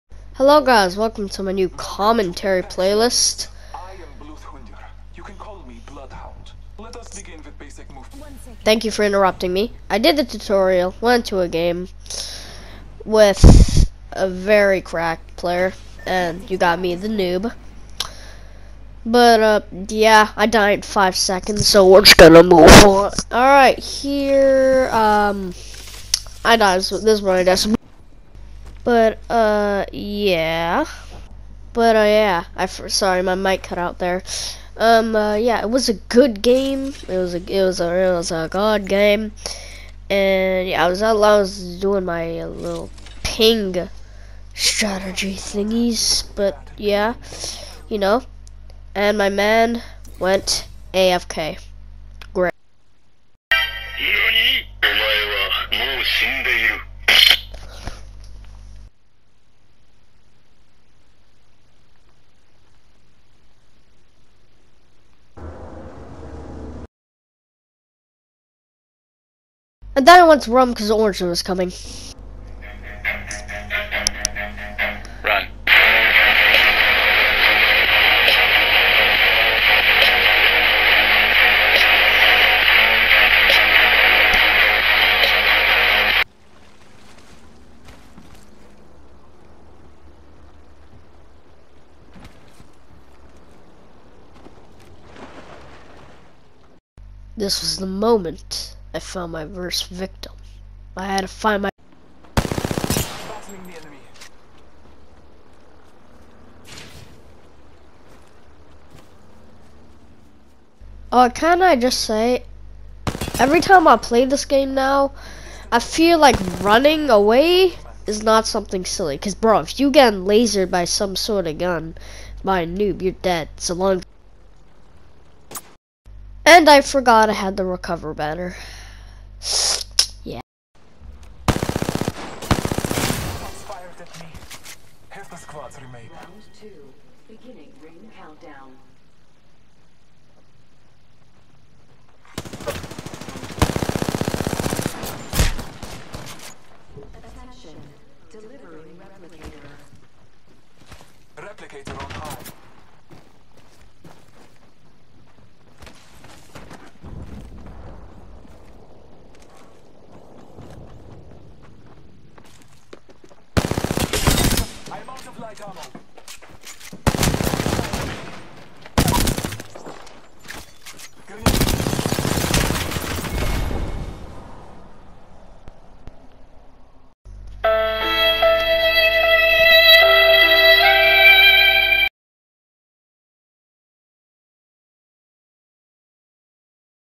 jb commenteary (episode 1)
Starting a new series where i play games and mostley do commenteary if you liked it subscribe you dont have to like that i dont care about likes so just subs  hope you enjoy(: